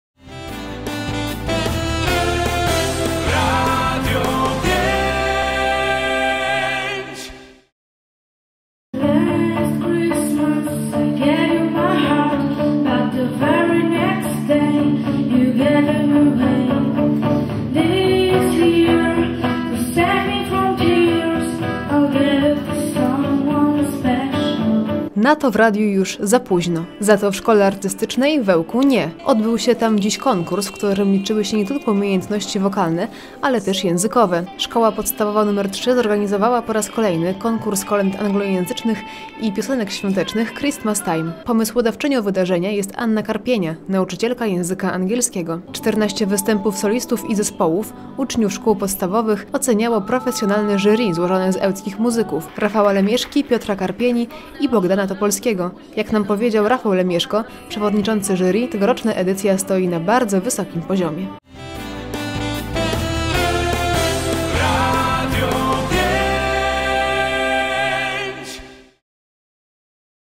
Christmas Time
W radiu jest już za późno na piosenki świąteczne, ale Szkole Artystycznej w Ełku nie! Odbył się tam dziś konkurs, w którym liczyły się nie tylko umiejętności wokalne, ale też językowe. Szkoła Podstawowa nr 3 zorganizowała po raz kolejny Konkurs Kolęd Anglojęzycznych i Piosenek Świątecznych "Christmas Time". Pomysłodawczynią wydarzenia jest Anna Karpienia, nauczycielka języka angielskiego. 14 występów solistów i zespołów - uczniów szkół podstawowych - oceniało profesjonalne jury złożone z ełckich muzyków -  Rafała Lemieszki, Piotra Karpieni i Bogdana Topolskiego.  Jak powiedział reporterce Radia 5 Rafał Lemieszko, przewodniczący jury, tegoroczna edycja stoi na bardzo wysokim poziomie.